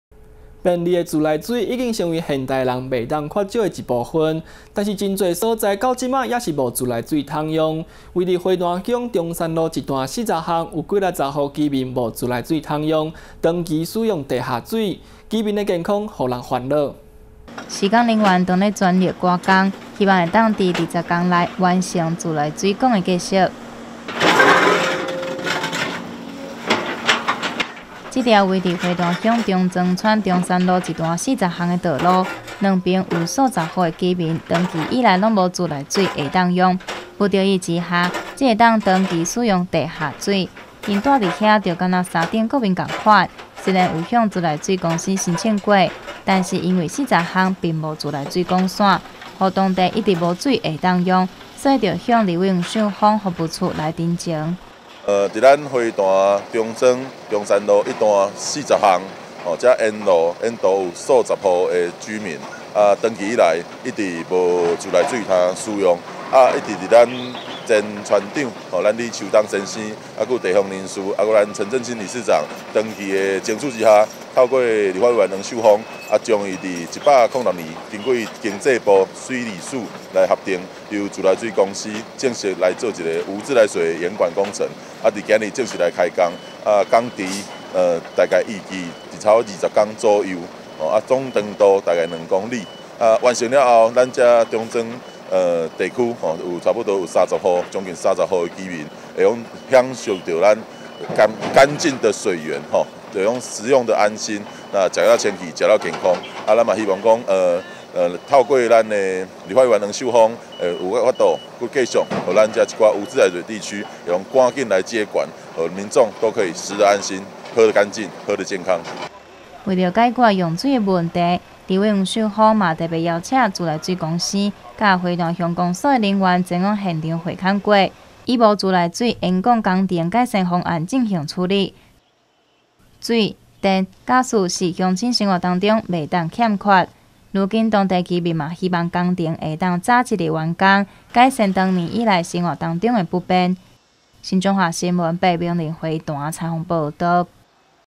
新彰化新聞20180614 花壇中庄架設自來水管 不再僅有地下水源
便利的自來水已經成為現代人不可或缺的一部分，但是很

多地方，至今還是無自來水可用，位於花壇鄉中山路一段

40巷，有數十戶居民無自來水可用，讓居民被迫長期飲用

地下水，危害居民健康。